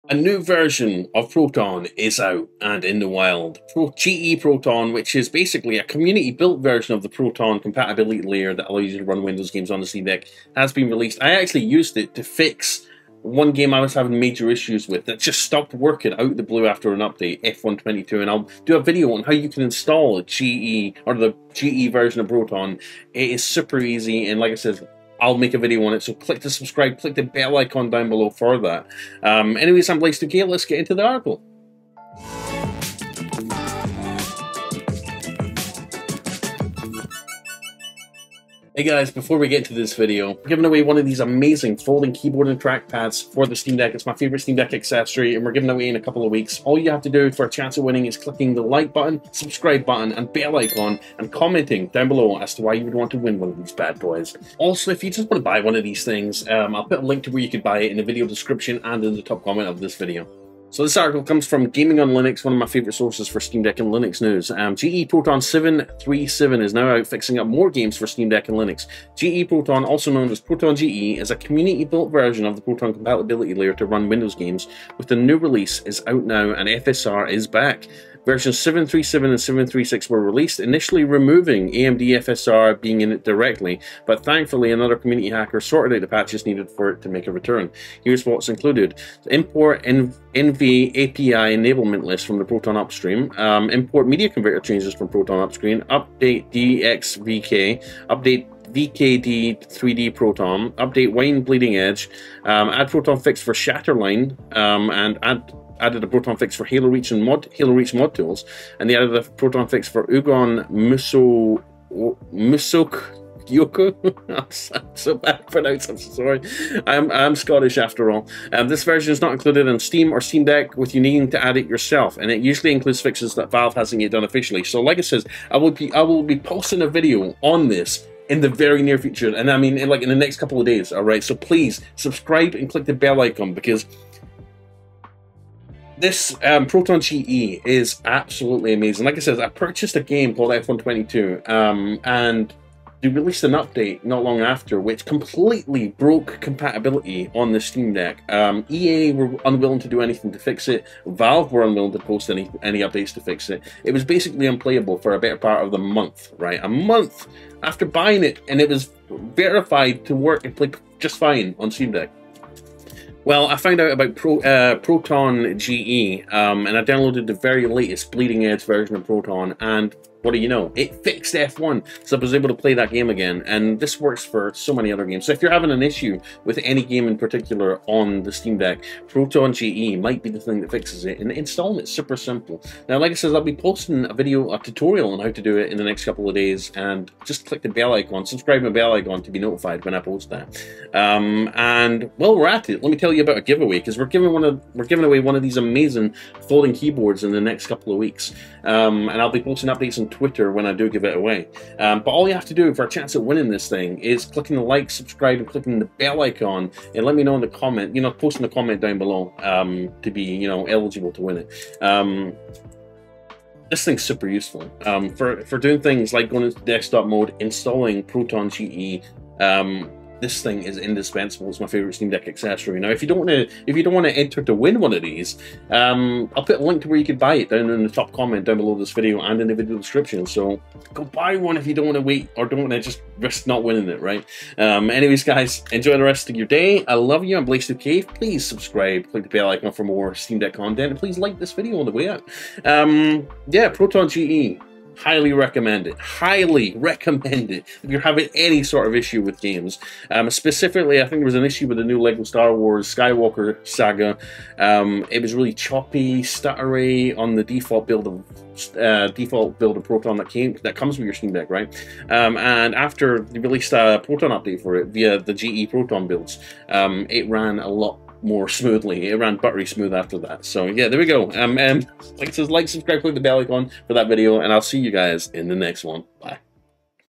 GE-Proton 7-37 - Lots of Game Compatibility fixes for Steam Deck!
A new update for Proton - the game compatibility layer for Windows software on Steam Deck is now available bringing with it the bleeding edge of game compatibility and support!

★ Foldable Keyboard/Trackpad for Deck
★ The BEST Steam Deck Accessories